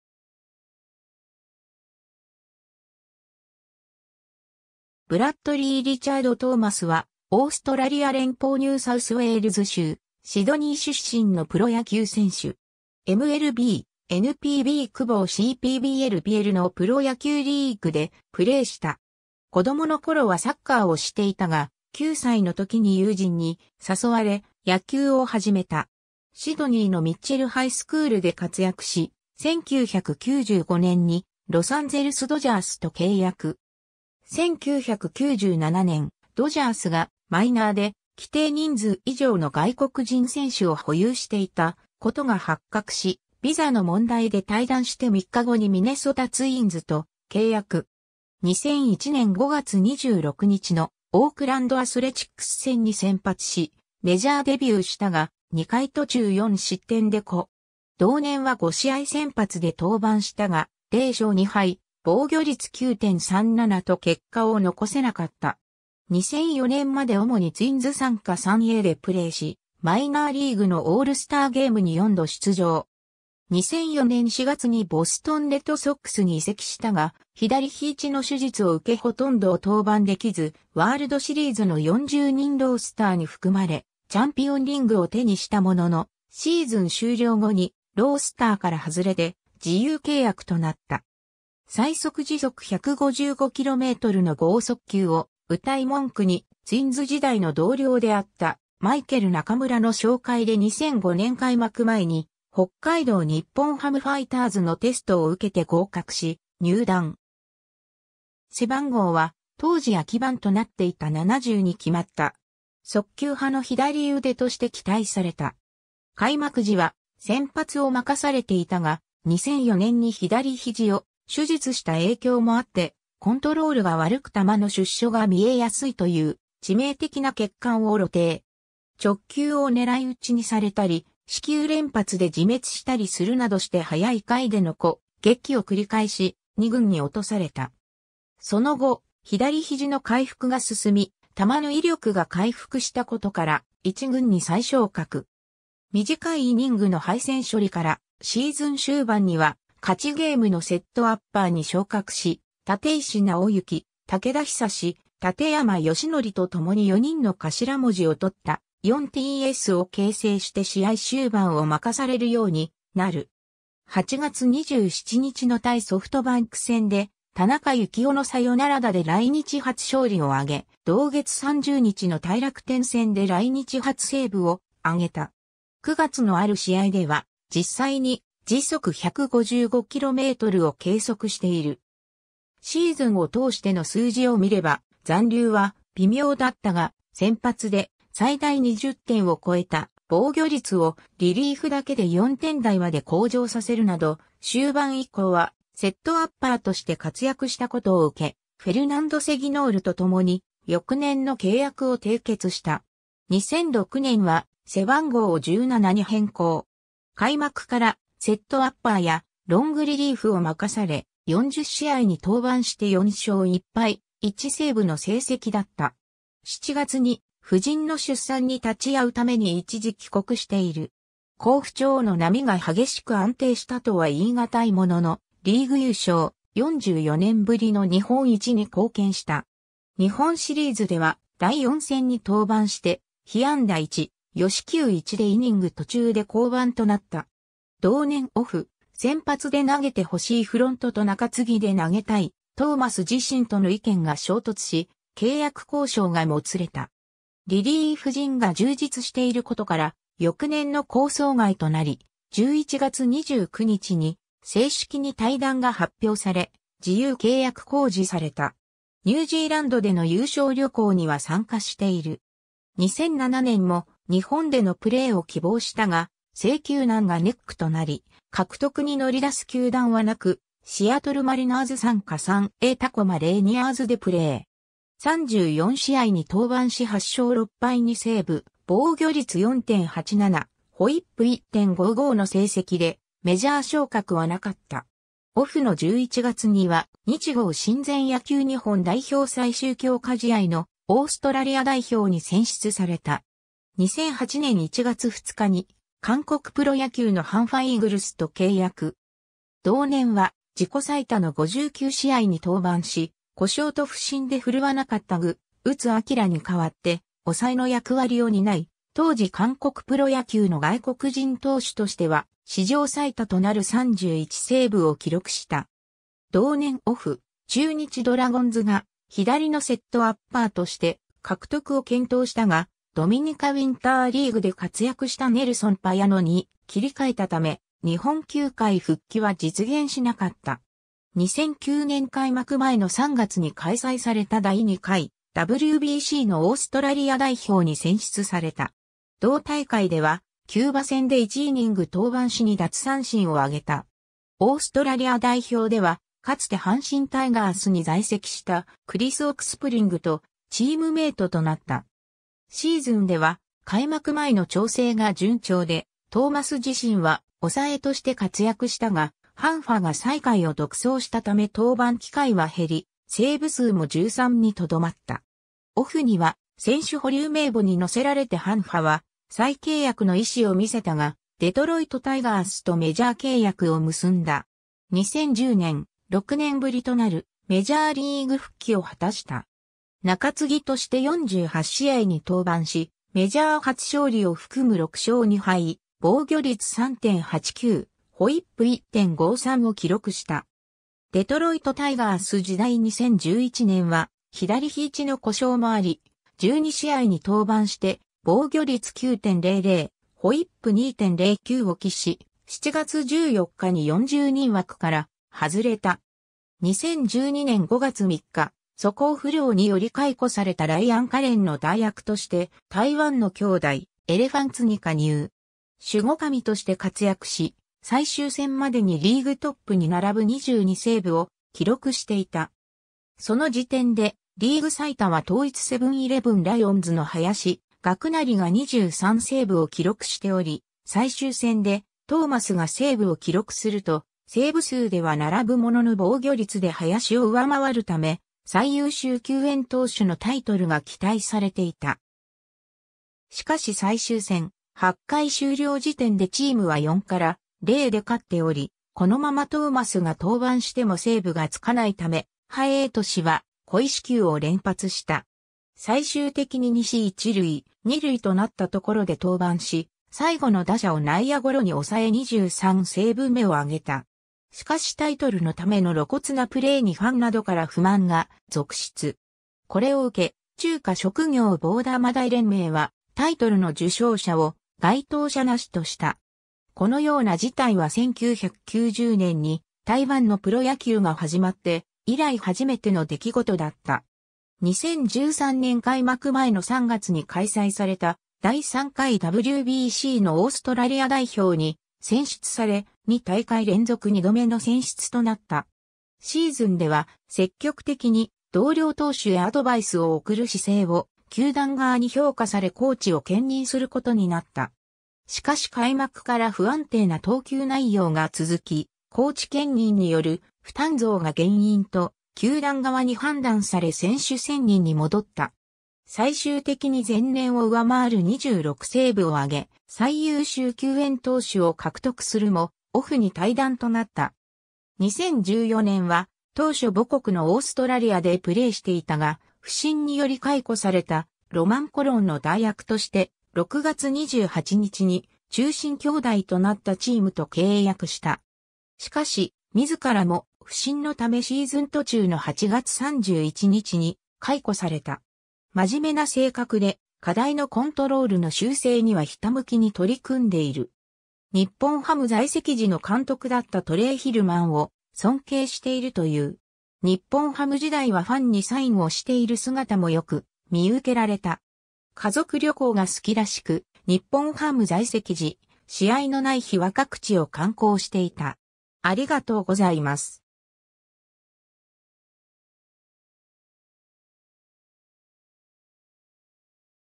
ブラッド・トーマス
ハンファ・イーグルス及びピングレ・イーグルスの選手
シドニー・ブルーソックスの選手
ワールド・ベースボール・クラシック・オーストラリア代表選手
ブラッドリー・リチャード・トーマス（Bradley Richard Thomas, 1977年10月12日 - ）は、オーストラリア連邦ニューサウスウェールズ州シドニー出身のプロ野球選手（投手）。
 MLB・NPB・KBO・CPBL・ABLのプロ野球リーグでプレーした。
 子どもの頃はサッカーをしていたが、9歳の時に友人に誘われ野球を始めた。
シドニーのミッチェル・ハイスクールで活躍し、1995年にロサンゼルス・ドジャースと契約。
 1997年、ドジャースがマイナーで規定人数以上の外国人選手を保有していたことが発覚し、ビザの問題で退団して3日後にミネソタ・ツインズと契約。
 2001年5月26日のオークランド・アスレチックス戦に先発しメジャーデビューしたが、2回途中4失点でKO。
同年は5試合先発で登板したが、0勝2敗、防御率9.37と結果を残せなかった。
 2004年まで主にツインズ傘下3Aでプレーし、マイナーリーグのオールスターゲームに4度出場。
2004年4月にボストン・レッドソックスに移籍したが、左肘の手術を受けほとんど登板できず、ワールドシリーズの40人ロースターに含まれチャンピオンリングを手にしたものの、シーズン終了後にロースターから外れて自由契約となった。
 最速155km/hの豪速球を謳い文句に、ツインズ時代の同僚であったマイケル中村の紹介で2005年開幕前に北海道日本ハムファイターズのテストを受けて合格し入団。
背番号は当時空き番となっていた70に決まった。
速球派の左腕として期待された。
 開幕時は先発を任されていたが、2004年に左肘を手術した影響もあって、コントロールが悪く球の出所が見えやすいという致命的な欠陥を露呈。
直球を狙い撃ちにされたり、四球連発で自滅したりするなどして早い回でのKO劇を繰り返し、二軍に落とされた。
その後、左肘の回復が進み、球の威力が回復したことから、一軍に再昇格。
短いイニングの敗戦処理から、シーズン終盤には勝ちゲームのセットアッパーに昇格し、立石尚行・武田久・建山義紀とともに4人の頭文字を取った「4Ts（フォーティーズ）」を形成して試合終盤を任されるようになる。
8月27日の対ソフトバンク戦で、田中幸雄のサヨナラ打で来日初勝利を挙げ、同月30日の対楽天戦で来日初セーブを挙げた。
9月のある試合では、実際に155 km/hを計測している。
シーズンを通しての数字を見れば残留は微妙だったが、先発で最大20点を超えた防御率をリリーフだけで4点台まで向上させるなど、終盤以降はセットアッパーとして活躍した事を受け、フェルナンド・セギノールと共に翌年の契約を締結した。
 2006年は背番号を17に変更。
開幕からセットアッパーやロングリリーフを任され、40試合に登板して4勝1敗1セーブの成績だった。
7月に夫人の出産に立ち会うために一時帰国している。
好不調の波が激しく安定したとは言いがたいものの、リーグ優勝、44年ぶりの日本一に貢献した。
日本シリーズでは、第4戦に登板して被安打1、与四球1でイニング途中で降板となった（自責点は0）。
同年オフ、先発で投げてほしいフロントと中継ぎで投げたいトーマス自身との意見が衝突し、契約交渉がもつれた。
リリーフ陣が充実していることから翌年の構想外となり、11月29日に正式に退団が発表され、自由契約公示された。
ニュージーランドでの優勝旅行には参加している。
 2007年も日本でのプレーを希望したが、制球難がネックとなり獲得に乗り出す球団はなく、シアトル・マリナーズ傘下3Aタコマ・レイニアーズでプレー。
34試合に登板し8勝6敗2セーブ、防御率4.87、WHIP1.55の成績でメジャー昇格はなかった。
オフの11月には「日豪親善 野球日本代表最終強化試合」のオーストラリア代表に選出された。
 2008年1月2日に、韓国プロ野球のハンファ・イーグルスと契約。
 同年は自己最多の59試合に登板し、故障と不振で振るわなかった具臺晟に代わって抑えの役割を担い、当時韓国プロ野球の外国人投手としては史上最多となる31セーブを記録...